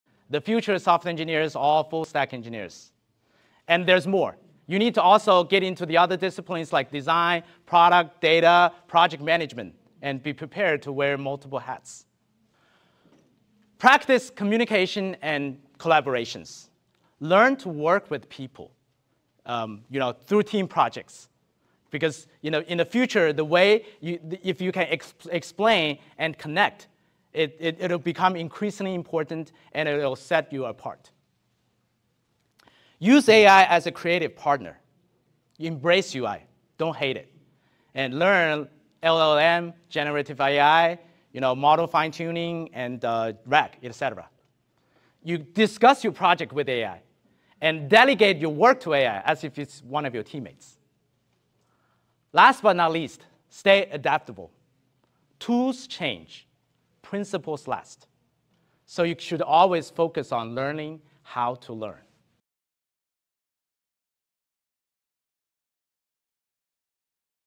The Future Isn’t Full-Stack engineers — It’s Full-Stack
The most impactful professionals aren’t staying in one lane; they’re connecting ideas across roles, learning from different disciplines, and working with AI as a creative partner.

This tedtalk clip by Yun Raymond Fu, COE Distinguished Professor at Northeastern University, offers a glimpse into that shift - from specialists to systems thinkers, from one path to many.